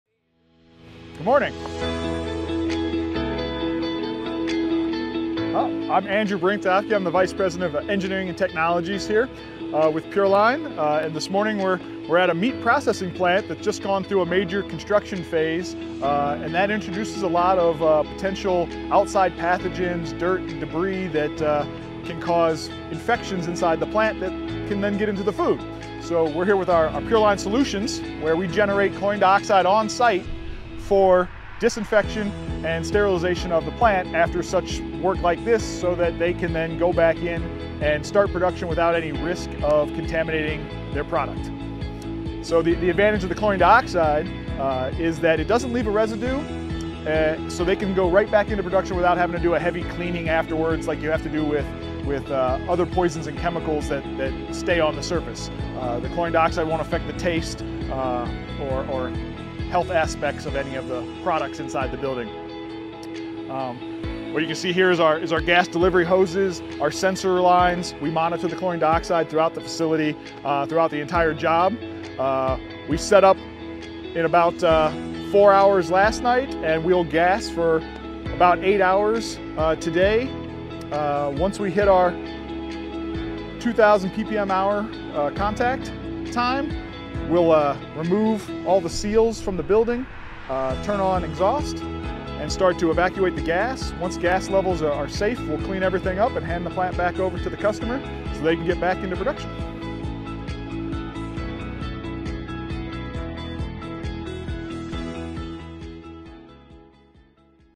Chlorine Dioxide Fumigation: Post-Construction Treatment for Food & Beverage Companies
Following the construction or the modification of a plant’s operations, consider a proven process to ‘reset the environment’ before operations begin. The result of any construction project can be a challenge to sanitize. It requires an extra procedure and adds safety in the fight against foodborne illnesses.

PureLine Solutions offers a chlorine dioxide fumigation treatment using our MobileClean technology which generates chlorine dioxide gas on-site, which is continually monitored in each treatment area, resulting in 6-log pathogen kills as verified through biological indicators and swab treatments.

Contact PureLine Solutions today to learn more about our Fumigation service for Food processors.